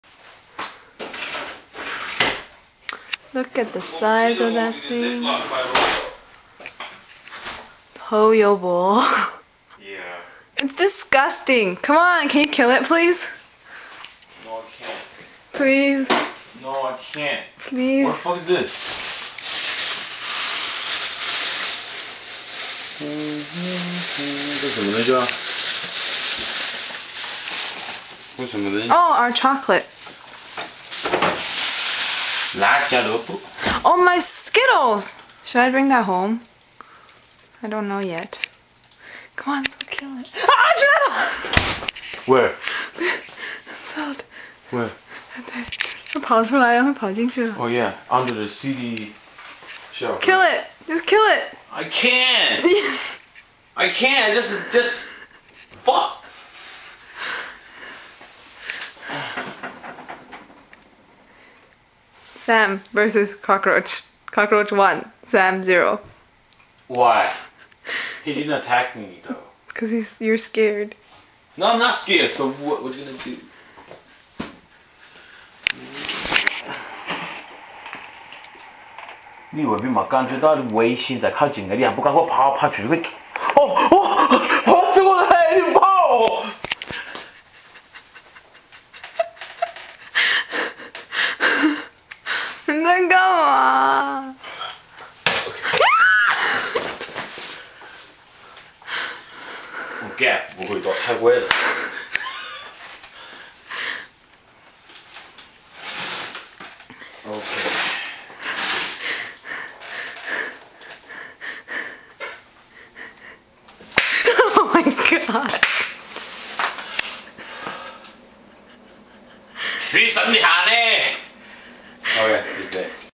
Sam killing Cockroach
Huge cockroach. scared us sh!$less